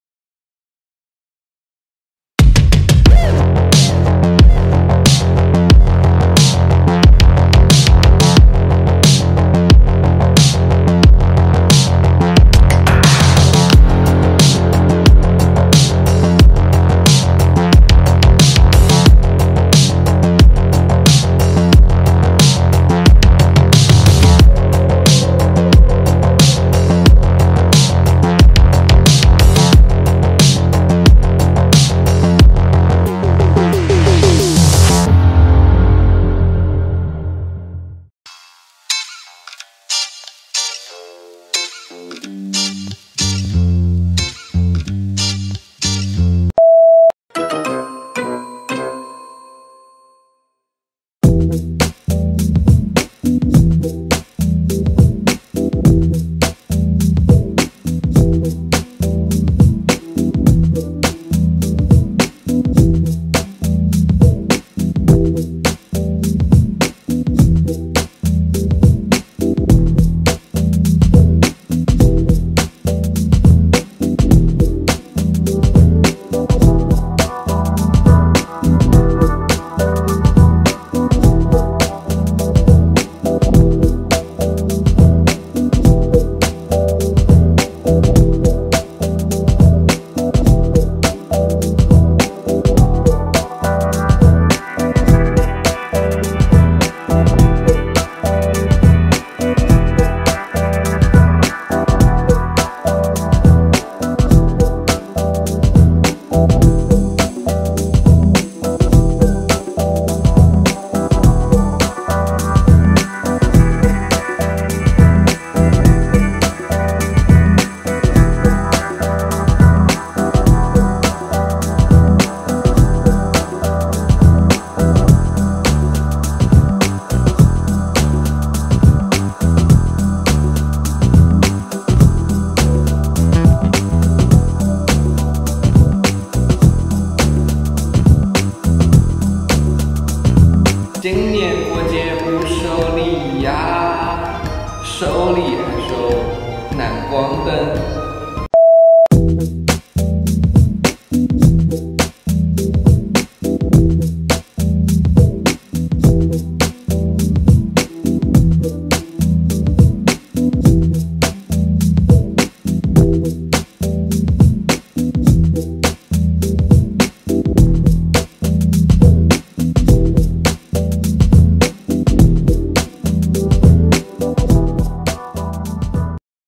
【南光Nanlite】—质感视觉大片！商业广告片案例《拍皮靴》
本期案例是一组黑白灰的高级时尚商业片
除了用灯光突出了皮靴的质感，也拍摄了几组突出人物的镜头。
心意满满，各位大大看完也点个发财的小手。
当然也欢迎三连加小南哥哥进组织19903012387